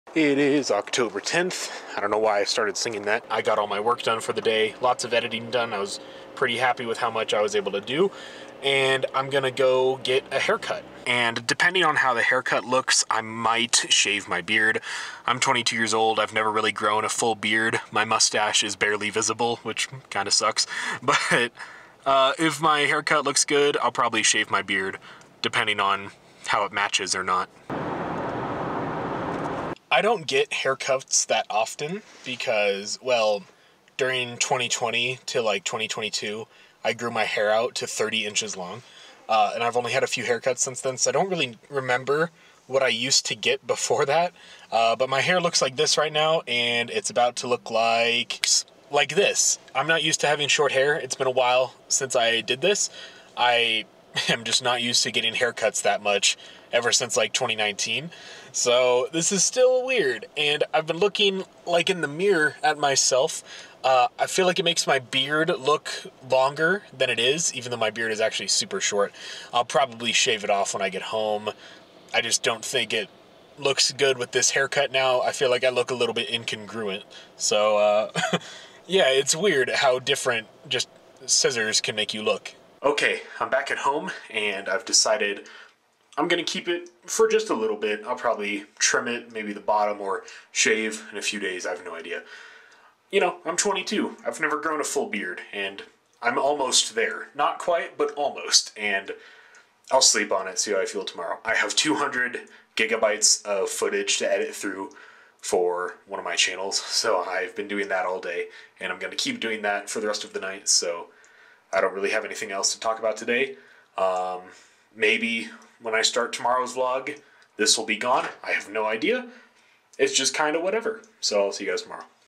I got a haircut
No idea what to do with my beard though